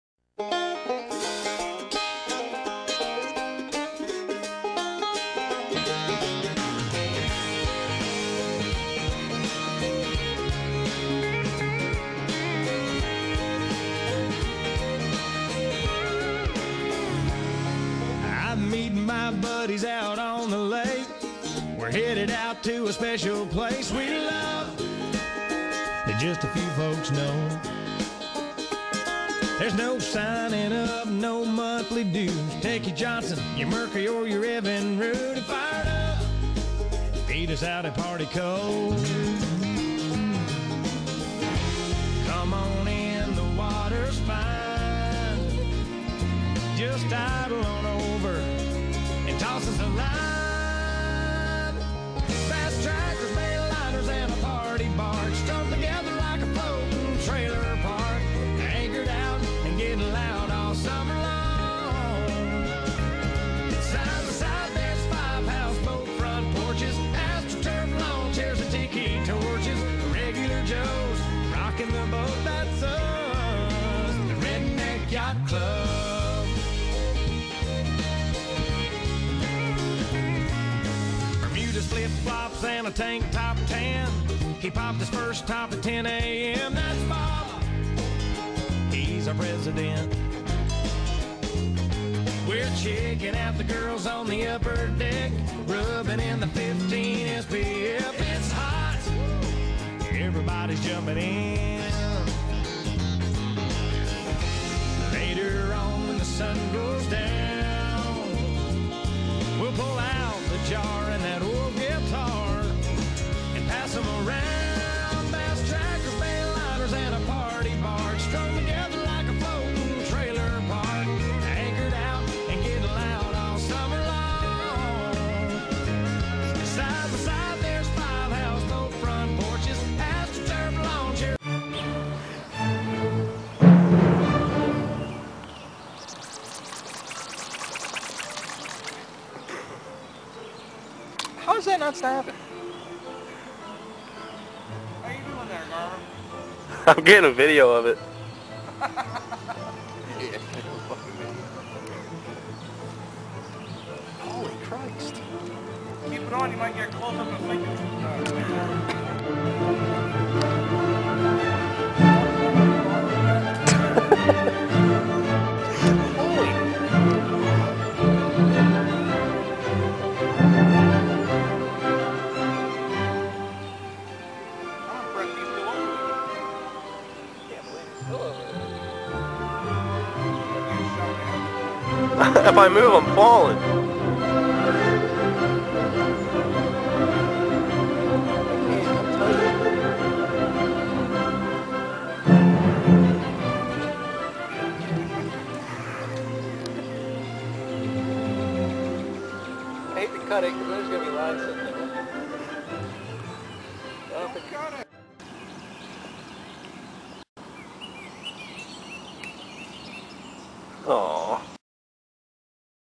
MONSTER FISH!!!
A few pictures from some of the fishing trips that a few of my buddies and I went on this past summer...Be sure to watch the last minute of the video for a classic fishing moment...hahaha...